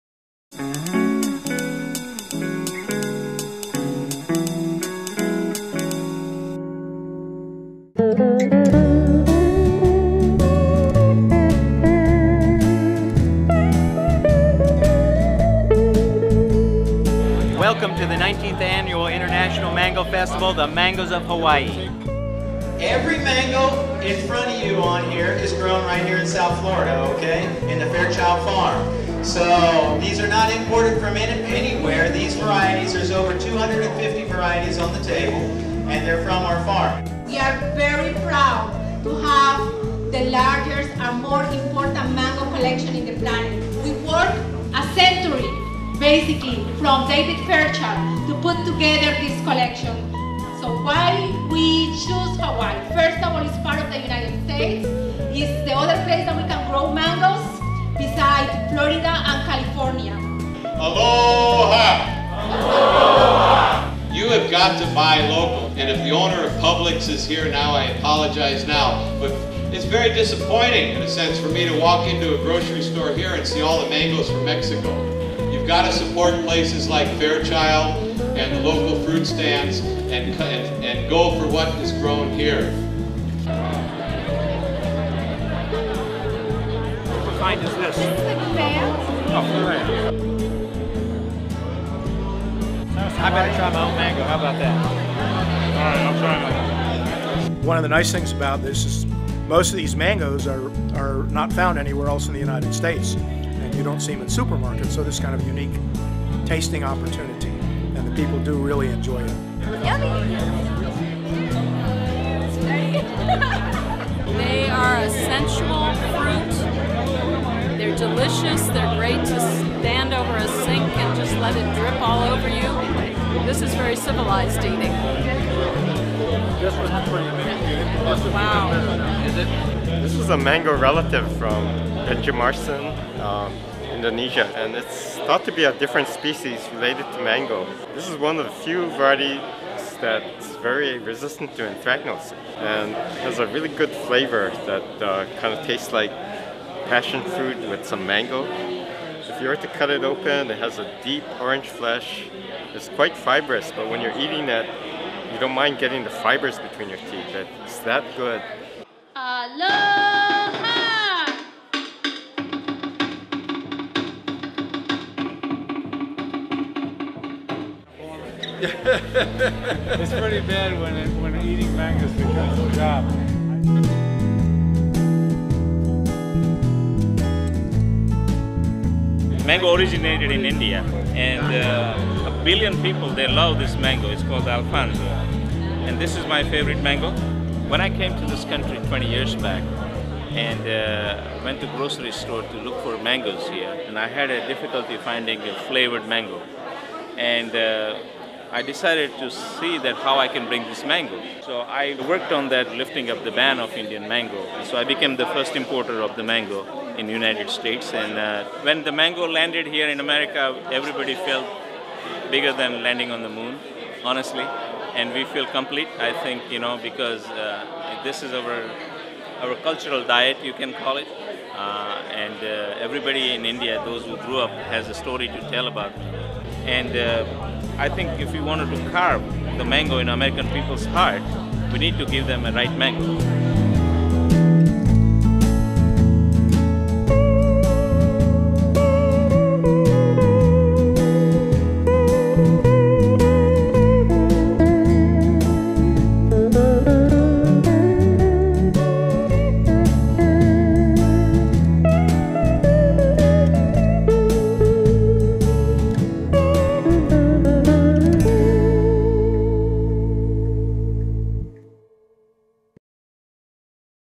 Video Bite: Mango Luau at Fairchild Tropical Garden in Miami
As we say farewell to another South Florida mango season, Brad Bryanand I created this Video Bite paean to a summer of truly amazing tropical fruit.
This was a knockout backyard mango season, judging by the plastic bags of mangoes that kept coming home and by the comments about mango overload I read online.
Brad and I got invited to the VIP mango luau that kicked off this year's International Mango Festival: The Mangoes of Hawaii.  This video produced by Miami Dish--the dish on all things edible & local in Miami.